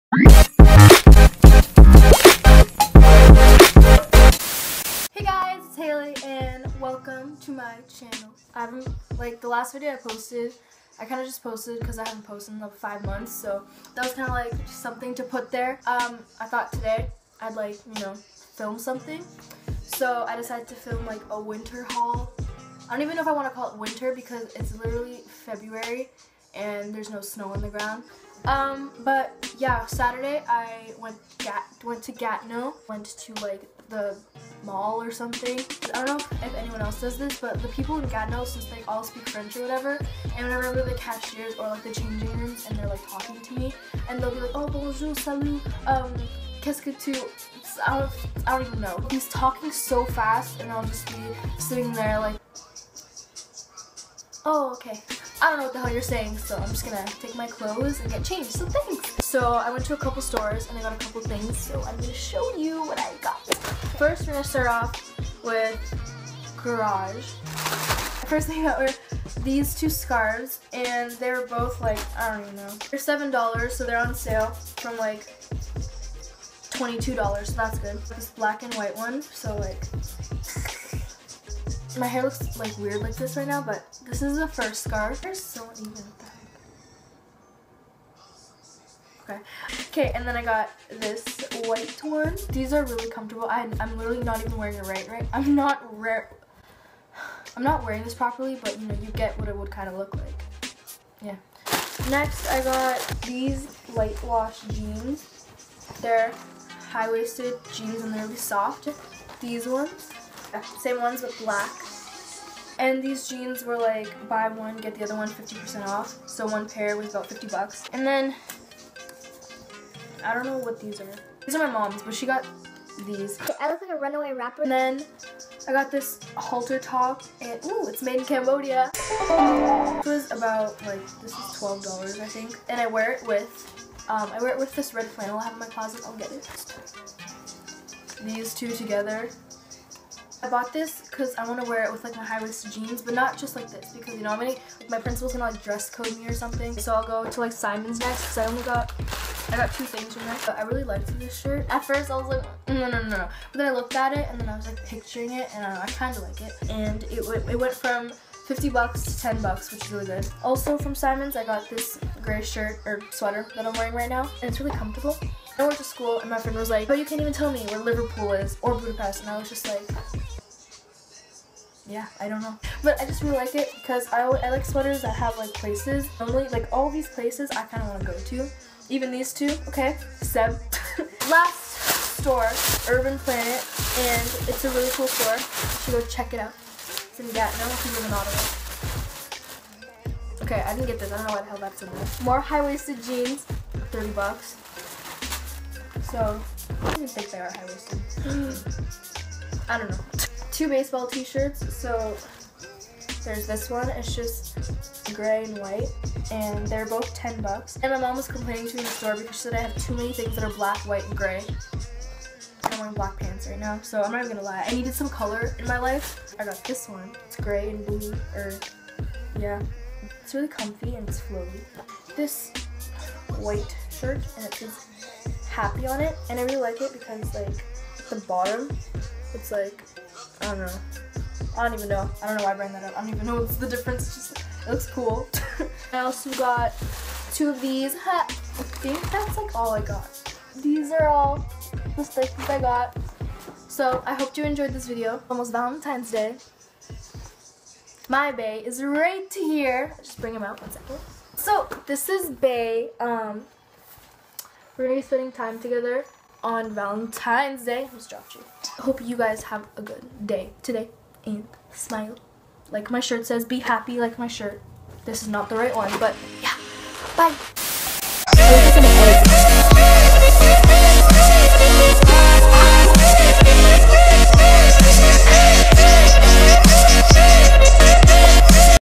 MINI WINTER HAUL! Garage, Simons, Urban Planet
hey guys !! so recently I went shopping in Gatineau and got a few things and made a haul. last time I tried to make a haul I looked like a fetus potato wearing jean jackets so no, lol but I tried, comment below what videos you want me to make !! xx